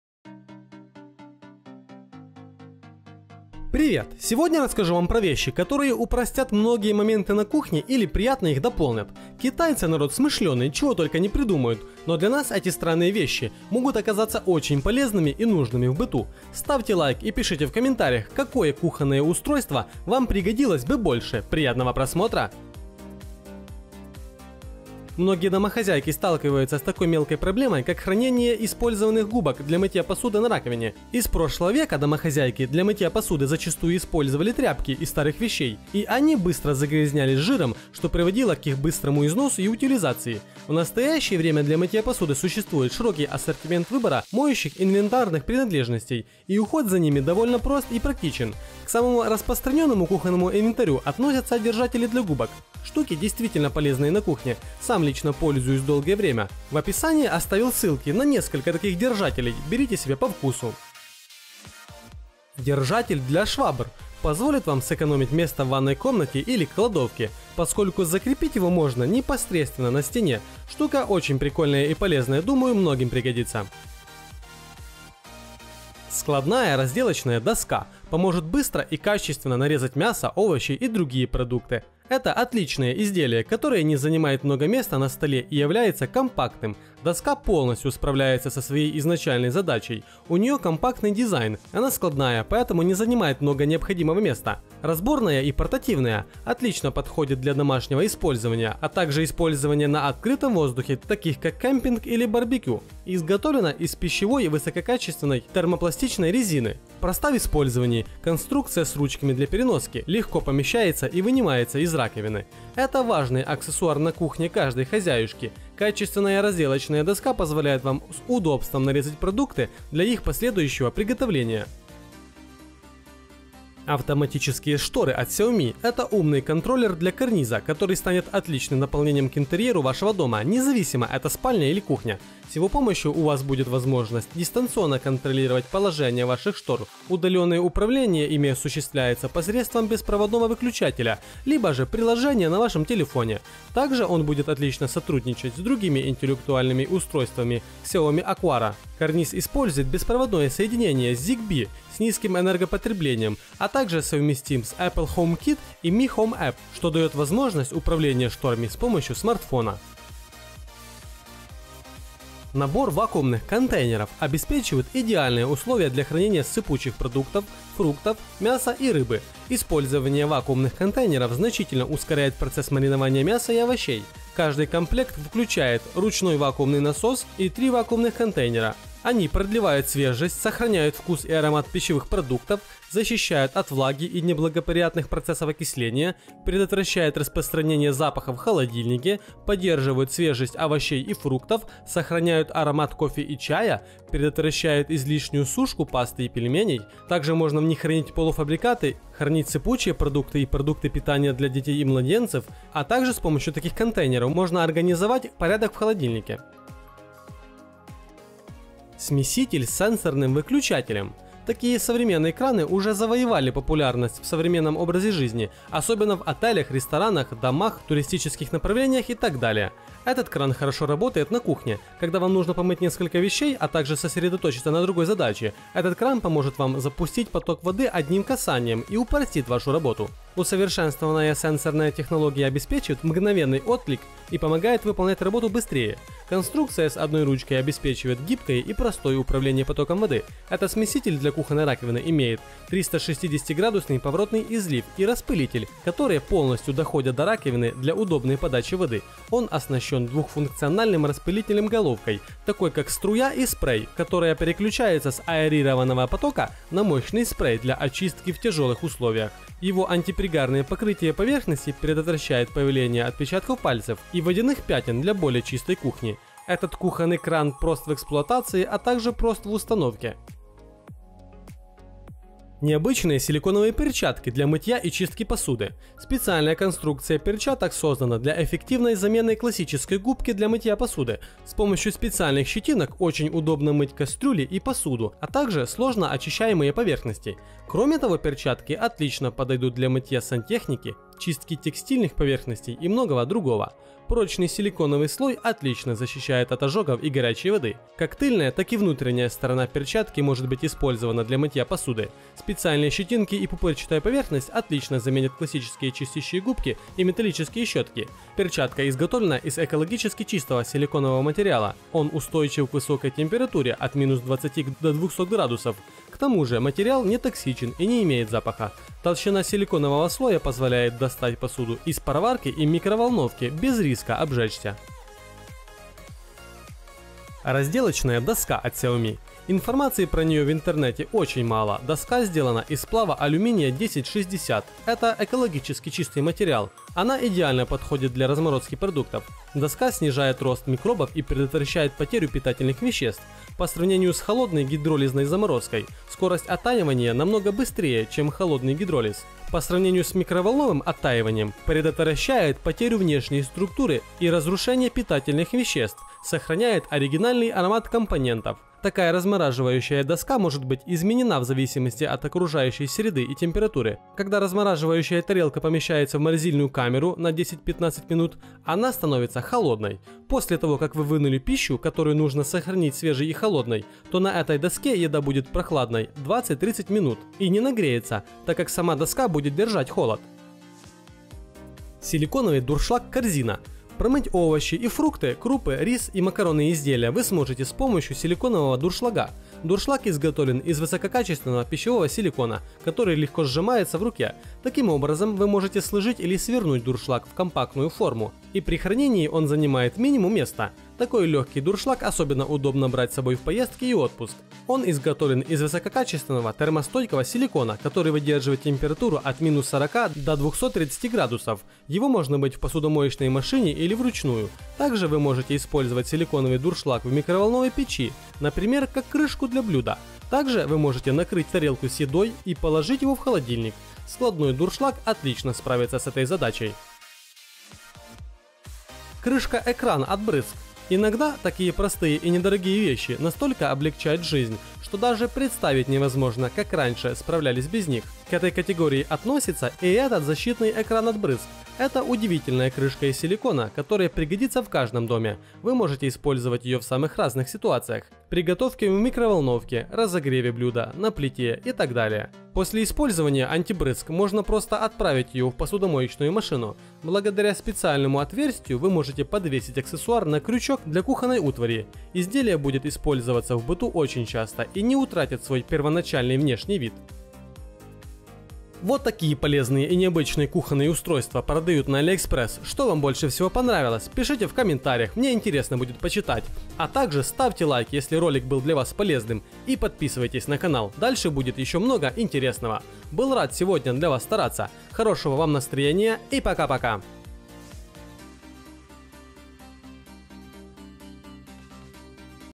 ВОТ ЭТО ДААААА! ОБАЛДЕТЬ! И ЭТО ДЛЯ УПРОЩЕНИЯ ГОТОВКИ НА КУХНЕ ДЛЯ ДОМА с ALIEXPRESS?
1. Держатель для губки

2. Держатель для швабр

3. Складная разделочная доска

4. Автоматические шторы

5. Набор вакуумных контейнеров

6. Смеситель с сенсорным выключателем

7. Силиконовые перчатки

8. Разделочная доска Xiaomi

9. Силиконовый дуршлаг-корзина

10. Крышка-экран от брызг